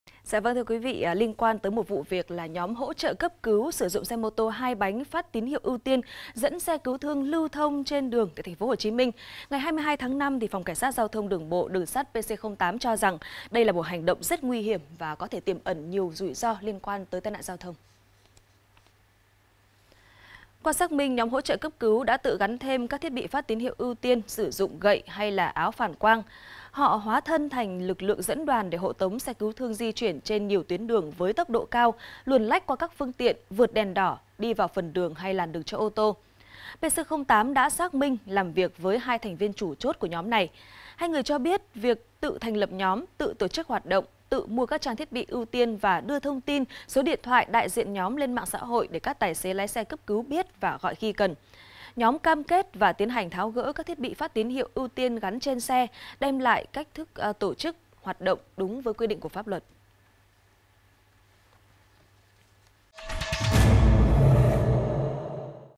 TP HCM: NHÓM HỖ TRỢ CẤP CỨU MỞ ĐƯỜNG XE CỨU THƯƠNG TIỀM ẨN NHIỀU TAI NẠN | VTC9
VTC9 | TP HCM: NHÓM HỖ TRỢ CẤP CỨU MỞ ĐƯỜNG XE CỨU THƯƠNG TIỀM ẨN NHIỀU TAI NẠN